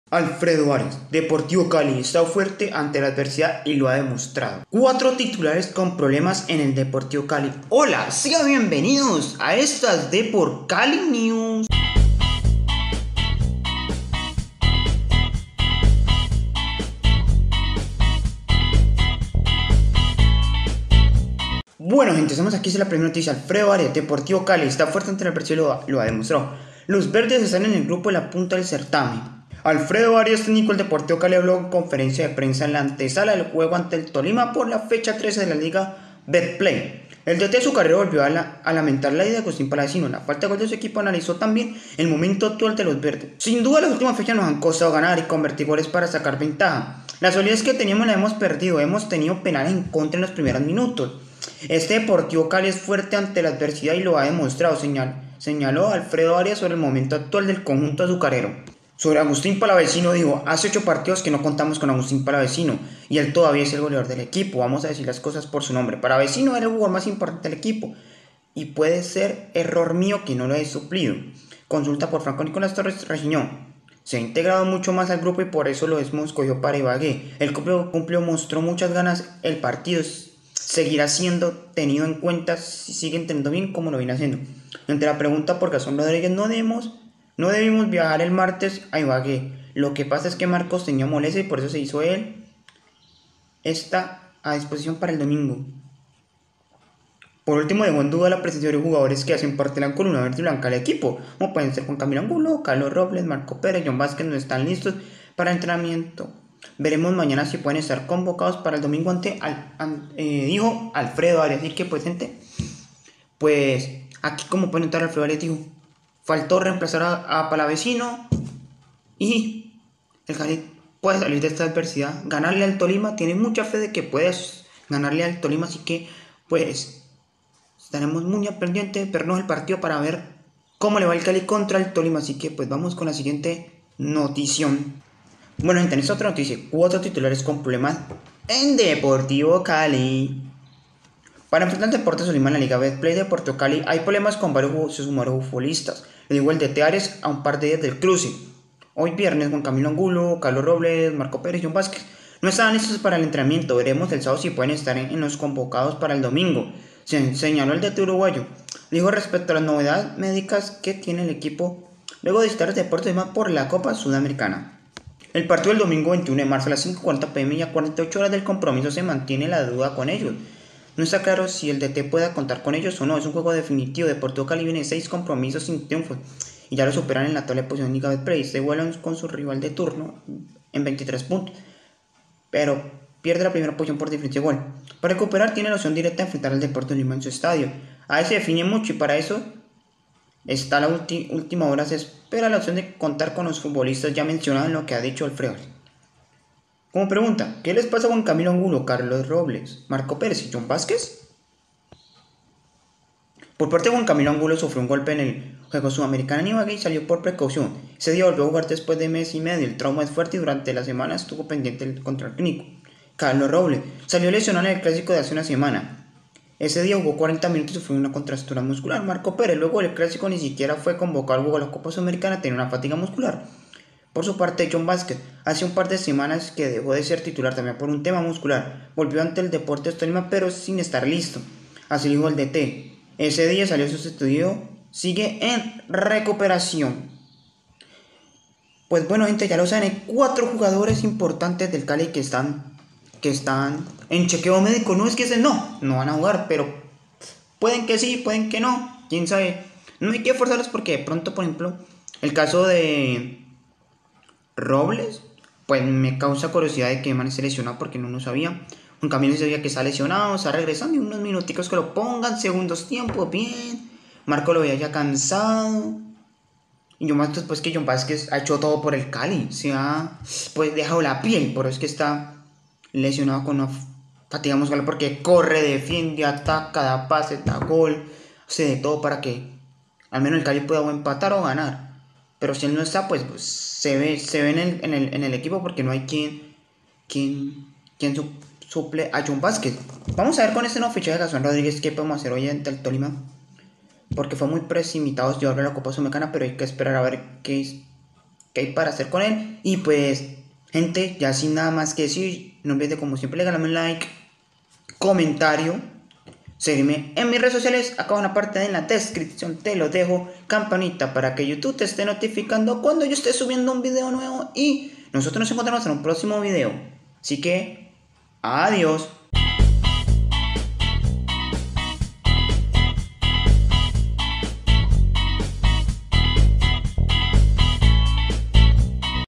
LAS 4 BAJAS DEL DEPORTIVO CALI ANTE EL TOLIMA, DT ARIAS HABLA SOBRE EL MAL MOMENTO DEL CALI PREVIA.
hola en este video te cuento los 4 jugadores que tiene el cali por baja ante el tolima, arias hbla sobre el mal momento del cali y dice que se arrepiente de no encontrar un sustituto a palavecino, esto y mucho mas en las depor cali news.